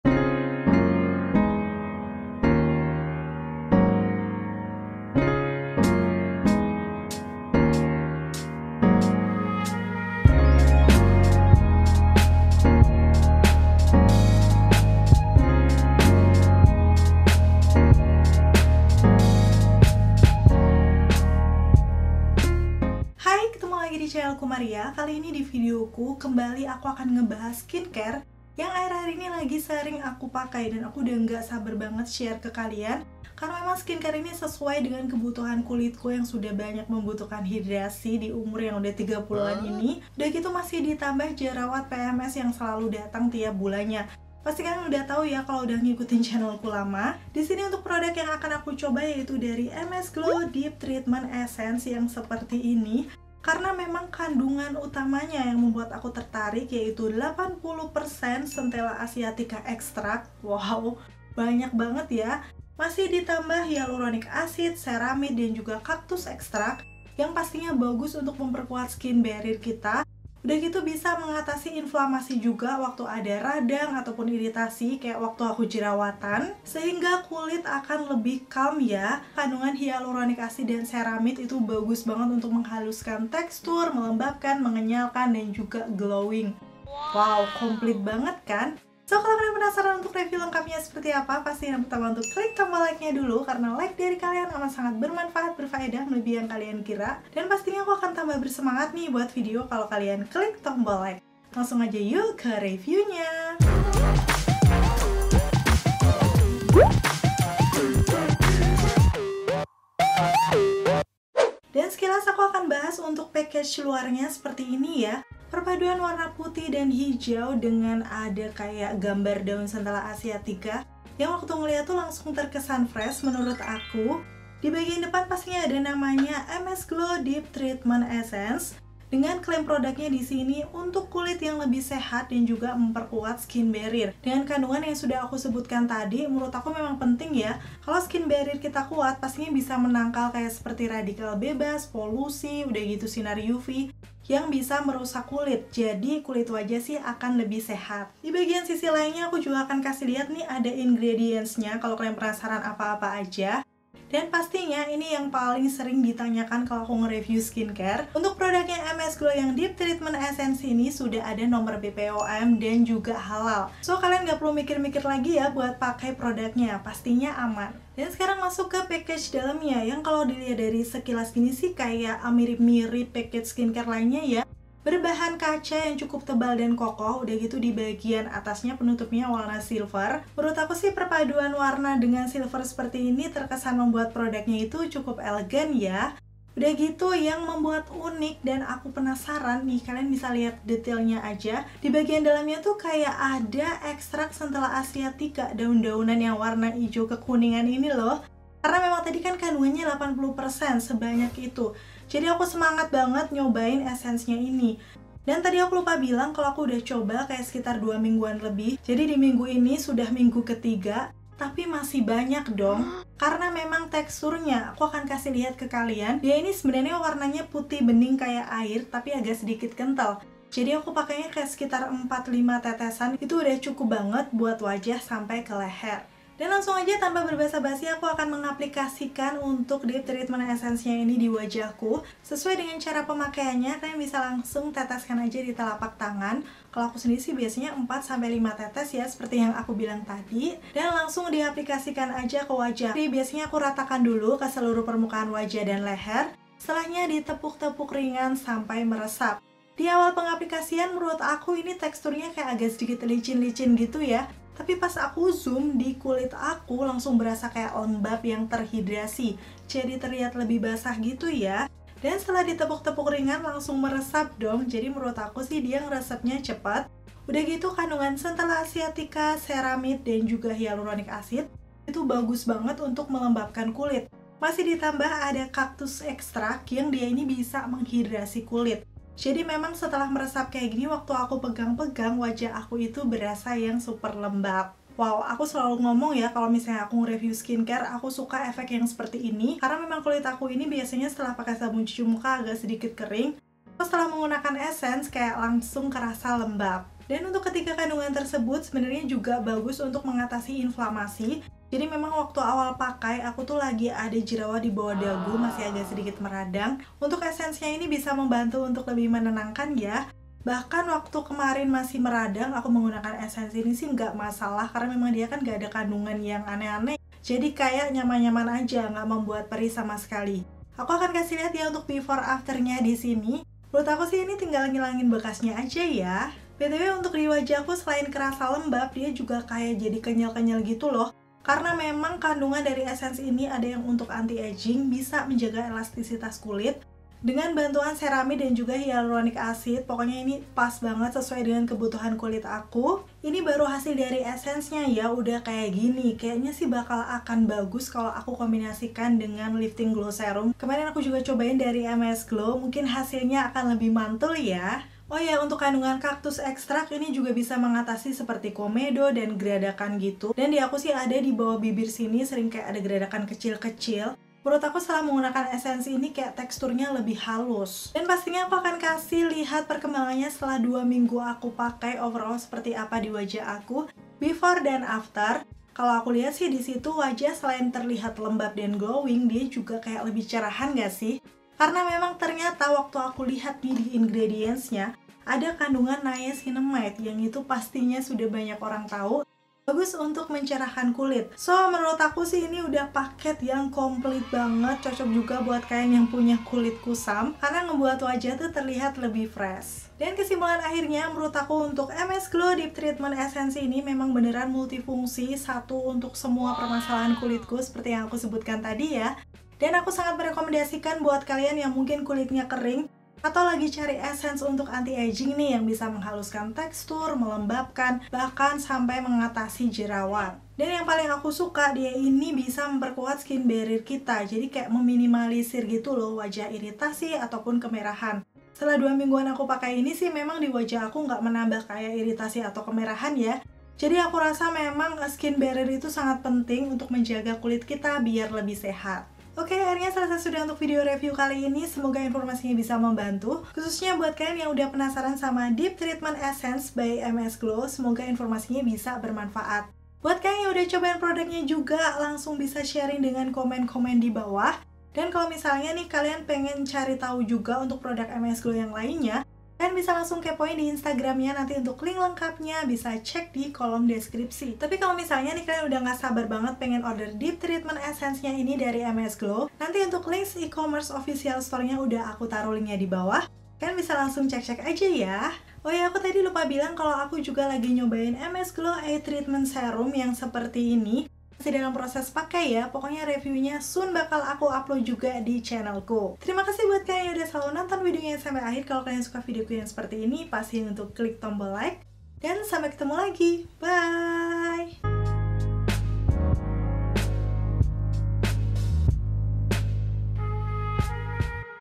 WAJAH GLOWING LEMBAB BEBAS JERAWAT ? MS GLOW DEEP TREATMENT ESSENCE PAKAI 2 MINGGU | Maria Soelisty
Hai😊
kembali aku akan bahas skincare routine ya setelah sekian lama dan aku udah ga sabar banget pengen share ke kalian Deep Treatment Essence ini yang fungsinya multifungsi banget. Karena dia ini ada kandungan Hyaluronic Acid , Ceramide, Cactus Extract yang bagus untuk menghidrasi kulit, jadi tetap lembab dan teksture juga halus. Termasuk komedo dan gradakan di dagu ku juga jadi lebih smooth gitu lho, suka banget😍
Dan masih banyak manfaat lainnya, yg pasti bisa 1 untuk semua, khususnya kalau kalian mau cari essence untuk anti aging .. nih juaranya karena beneran ada extraxt daun Centella Asiatica dengan kandungan 80%, wow🧐
pasti penasaran kan ? sabar yes, up sore ini di channel : @MariaSoelisty , enjoy , semoga bermanfaat❤️

PRODUK :
MS Glow Deep Treatment Essence
netto : 100 ml

Q&A :
- Ka, tipe kulitnya apa?
👧 tipe kulitku kombinasi, area TZone berminyak, area lain normal ke kering
- Ka, skintonenya apa?
👧 skintone medium aga kuning langsat
- Ka, pakai kamera apa?
👧 Canon EOS M 50
- Ka,edit video pake aplikasi apa?